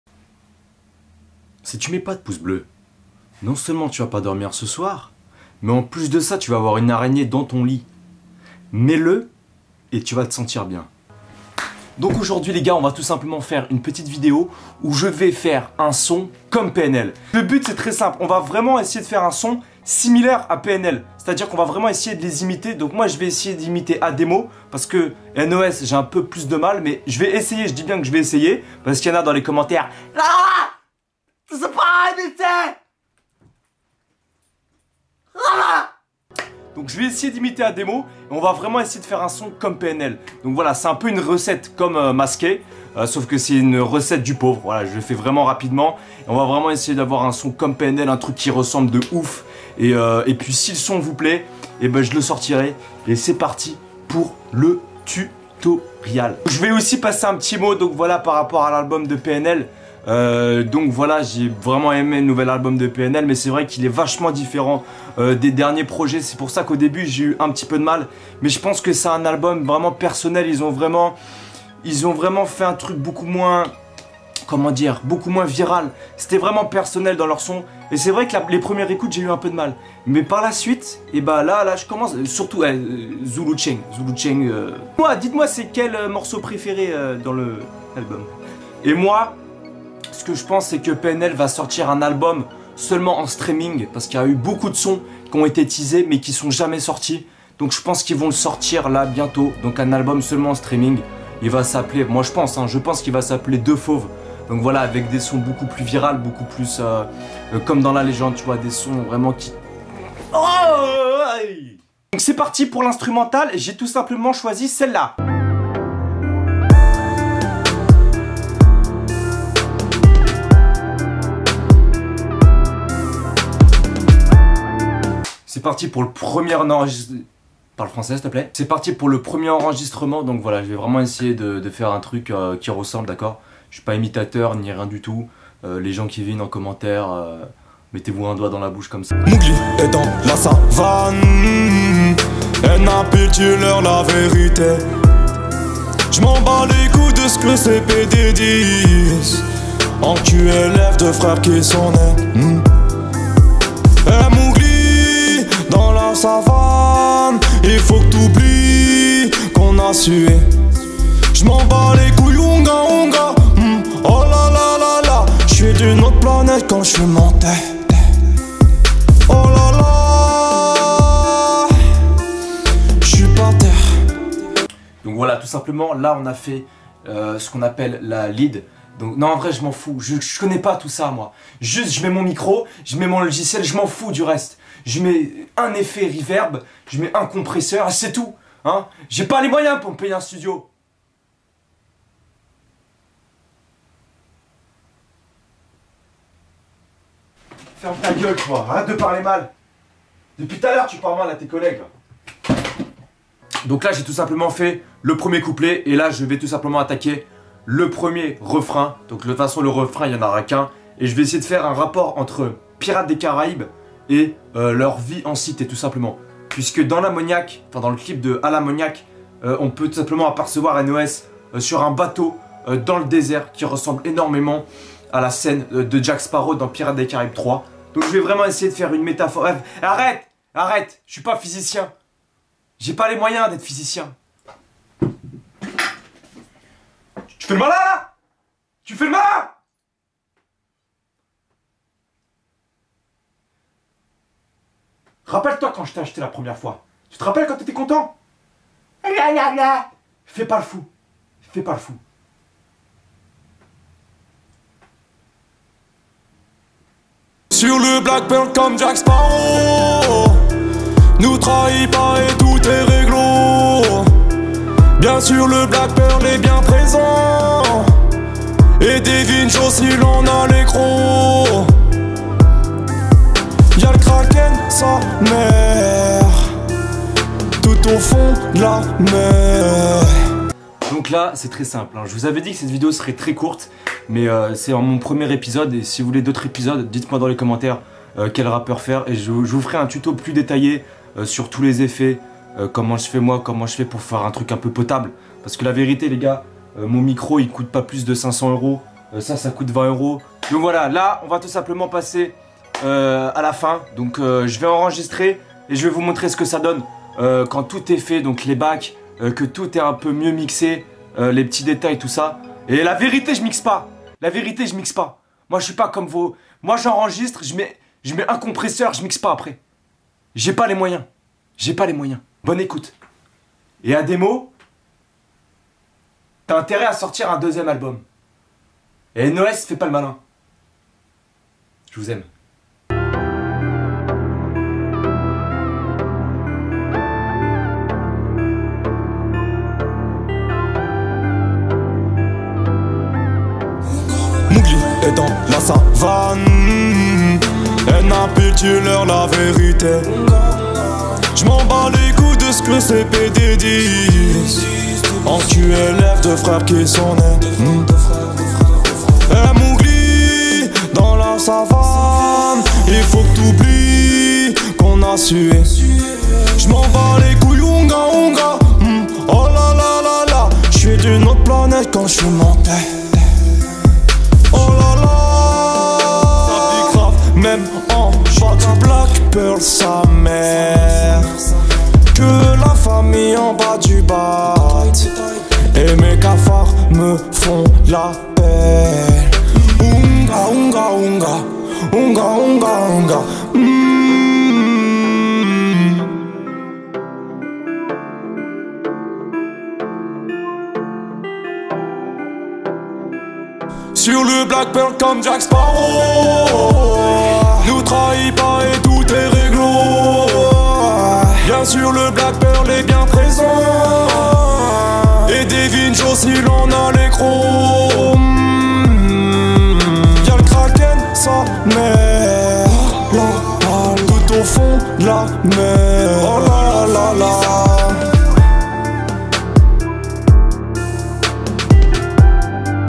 COMMENT FAIRE UNE MUSIQUE COMME PNL ?
Petite  vidéo rapide je tente de faire du pnl.